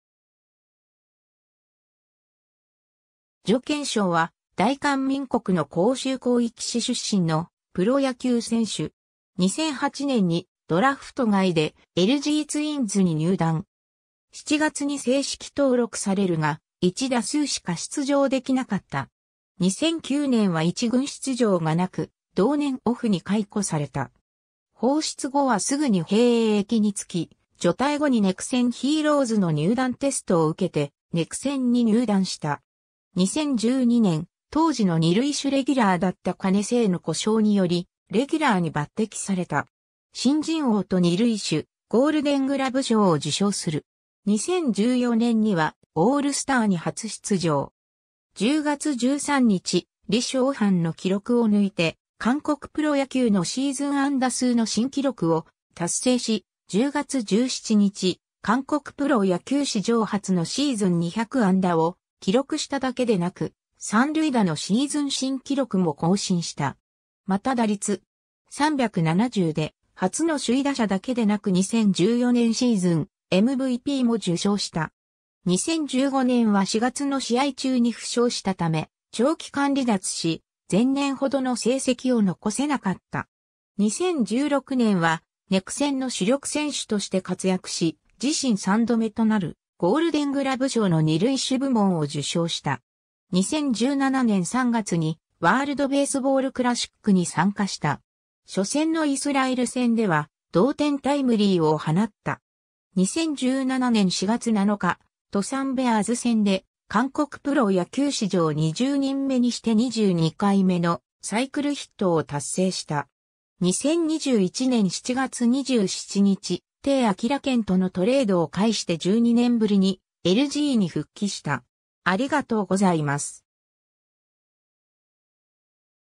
徐建昌
ワールド・ベースボール・クラシック韓国代表選手
キウム・ヒーローズ及びその前身球団の選手
徐 建昌（ソ・ゴンチャン、서건창、1989年8月22日 - ）は、大韓民国の光州広域市出身のプロ野球選手（内野手）。
 2008年にドラフト外でLGツインズに入団。
7月に正式登録されるが、1打数しか出場できなかった。
 2009年は一軍出場がなく、同年オフに解雇された。
 放出後はすぐに兵役に就き、除隊後にネクセン・ヒーローズの入団テストを受けてネクセンに入団した。
 2012年、当時の二塁手レギュラーだった金旼成の故障によりレギュラーに抜擢された。
新人王と二塁手ゴールデングラブ賞を受賞する。
 2014年にはオールスターに初出場。
10月13日、李鍾範の記録を抜いて韓国プロ野球のシーズン安打数の新記録(197)を達成し、10月17日、韓国プロ野球史上初のシーズン200安打を記録した(最終的には201安打)だけでなく、三塁打のシーズン新記録(17)も更新した（安打数は2019年にホセ・ミゲル・フェルナンデスが更新）。
また打率.370で初の首位打者だけでなく2014年シーズンMVPも受賞した。
 2015年は4月の試合中に負傷したため長期間離脱し、前年ほどの成績を残せなかった。
 2016年はネクセンの主力選手として活躍し、自身3度目となるゴールデングラブ賞の二塁手部門を受賞した。
 2017年3月にワールドベースボールクラシックに参加した。
初戦のイスラエル戦では同点タイムリーを放った。
 2017年4月7日、斗山ベアーズ戦で韓国プロ野球史上20人目にして22回目のサイクルヒットを達成した。
 2021年7月27日、鄭粲憲とのトレードを介して12年ぶりにLGに復帰した。